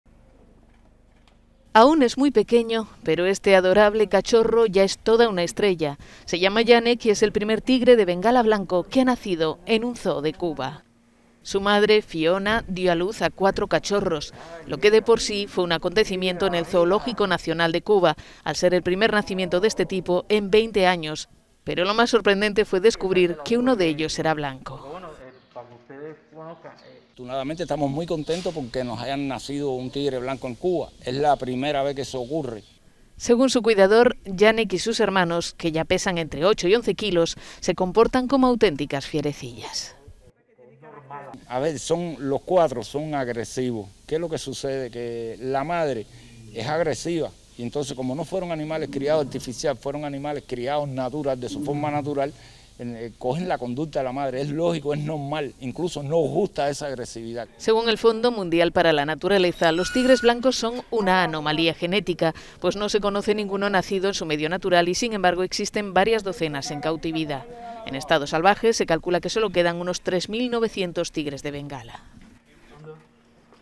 Nace en Cuba el primer tigre de Bengala blanco
Nace en Cuba el primer tigre de Bengala blanco. Se llama Yanek y es una auténtica fierecilla, según su cuidador. La madre tuvo cuatro crías, los primeros tigres de Bengala nacidos en Cuba en 20 años